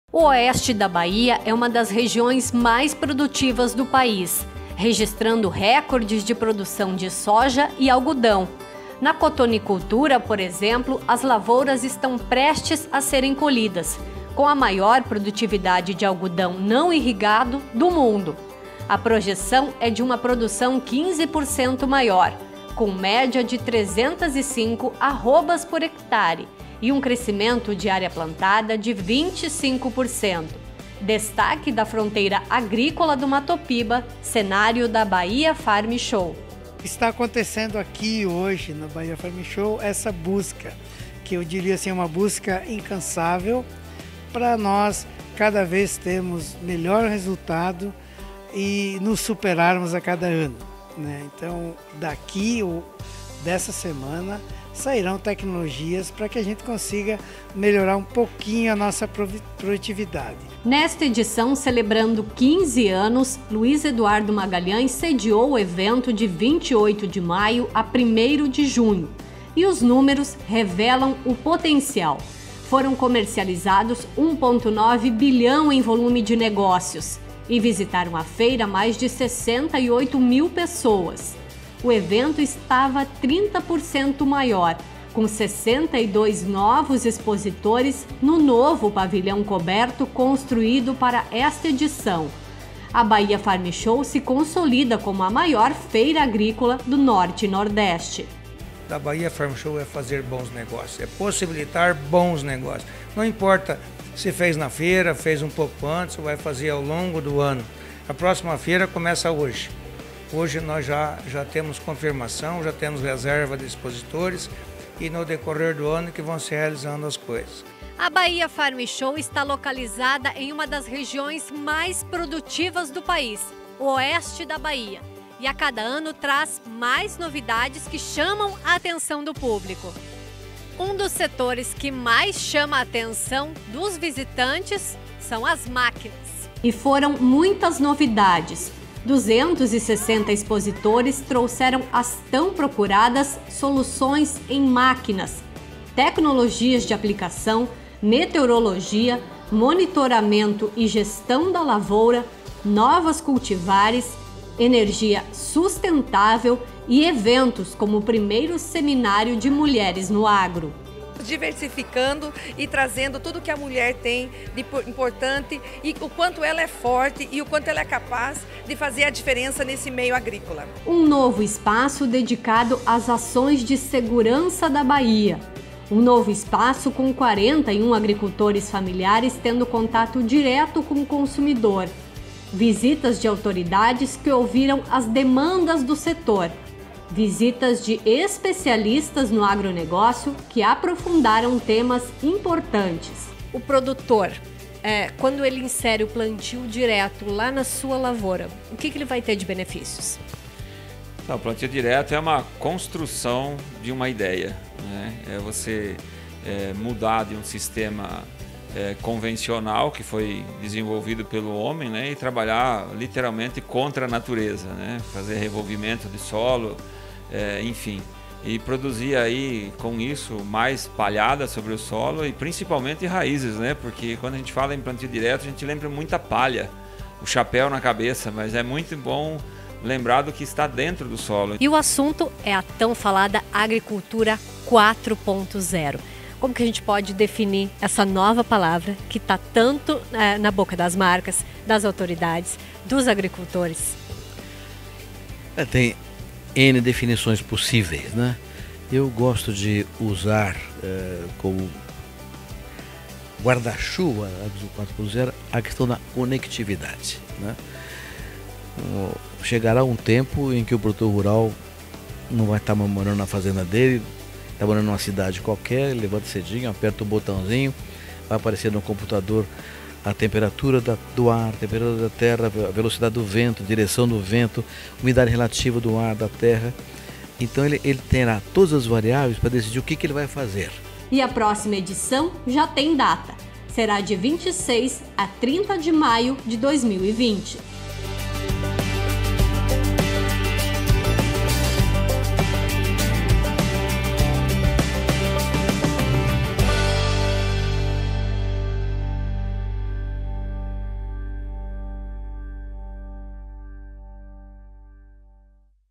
Bahia Farm Show: R$ 1.9 bi em negócios
As tecnologias, lançamentos, palestras técnicas fizeram a edição histórica de 15 anos da Bahia Farm Show ter recorde de faturamento e público. Confira as soluções e o retrato da safra de algodão que está começando na região.